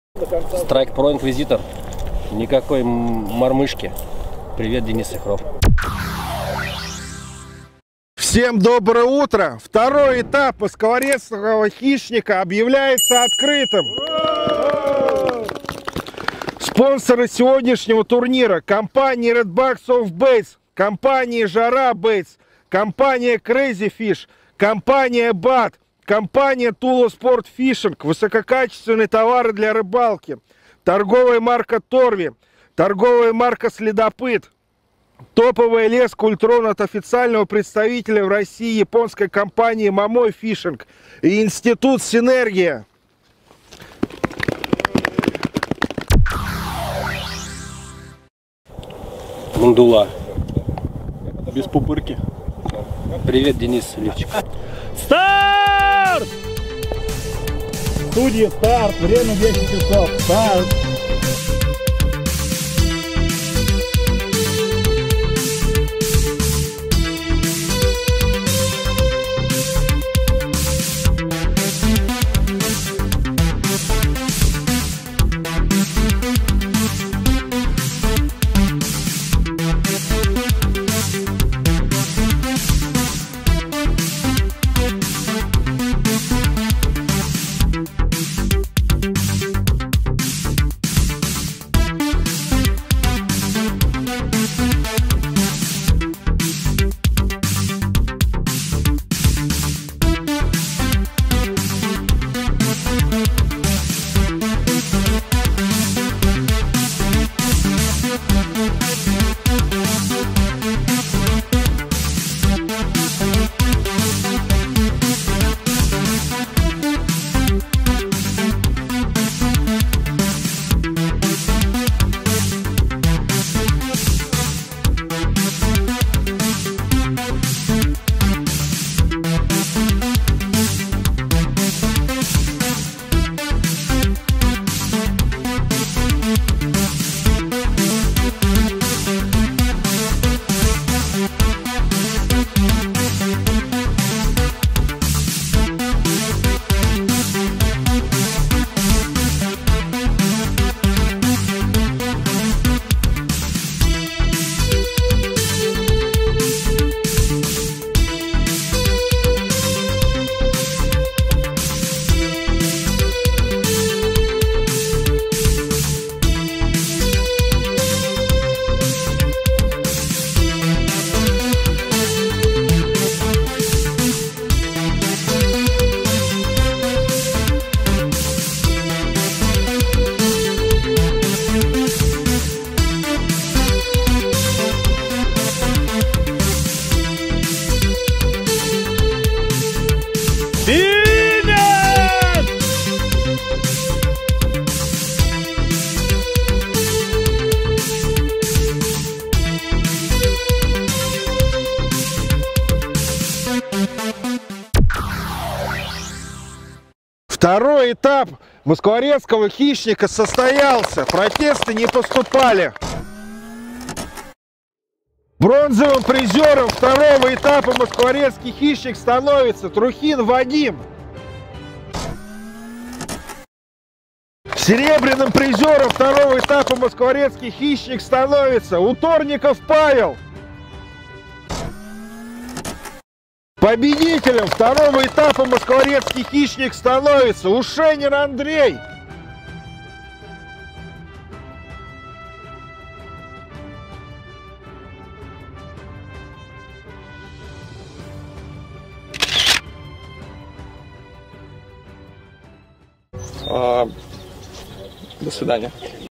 «Москворецкий хищник» 26122020
Второй этап рыболовного соревнования «Москворецкий хищник» прошел 26-го декабря 2020 года на берегу реки Москва Раменский район, д.Кулаково. Призы от ТМ TORVI